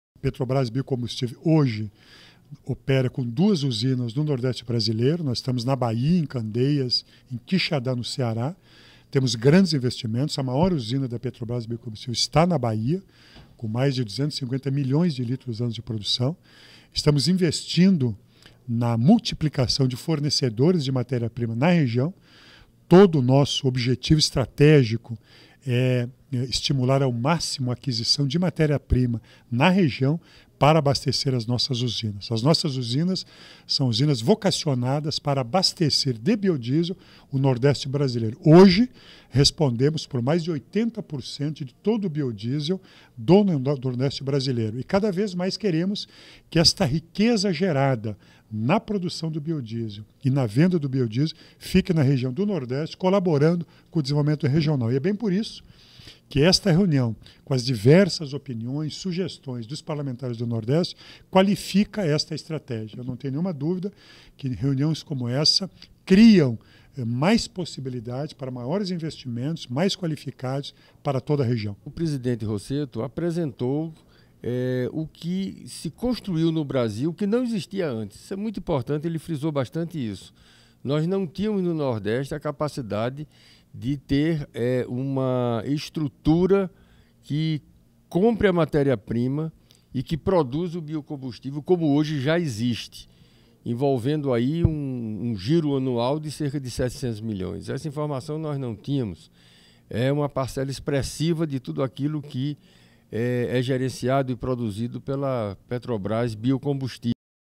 Bancada do Nordeste com a Petrobras Biocombustível
Bancada do Nordeste realiza reunião com o presidente da Petrobras Biocombustível Miguel Rossetto.